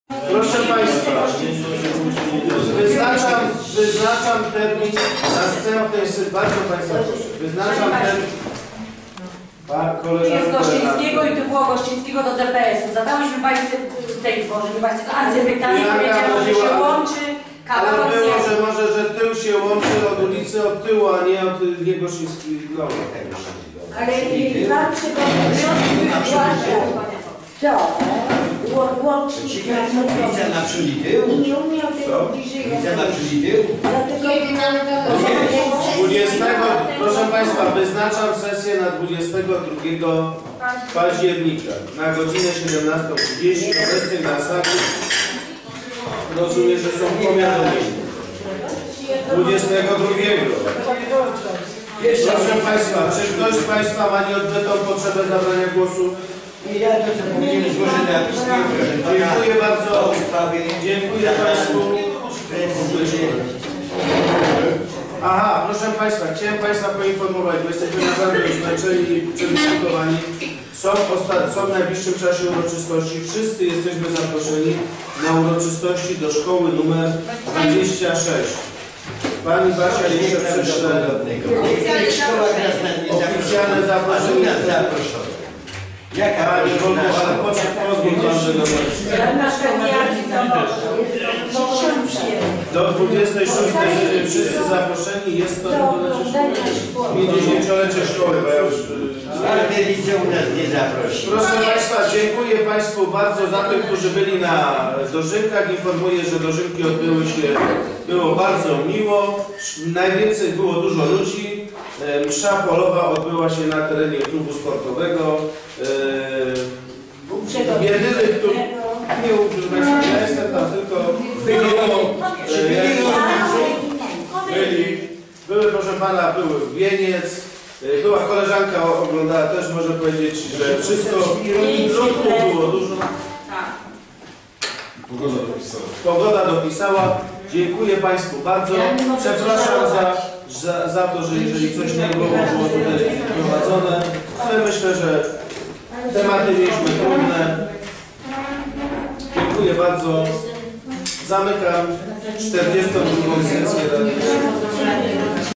RD_XIII_24-09-2013_22 Oświadczenia, komunikaty, wolne wnioski, zamkniecie sesji
Obrady XLII sesji Rady Dzielnicy XIII Podgórze z dn. 24 wrześnie 2013.

Oświadczenia, komunikaty, wolne wnioski, zamknięcie sesji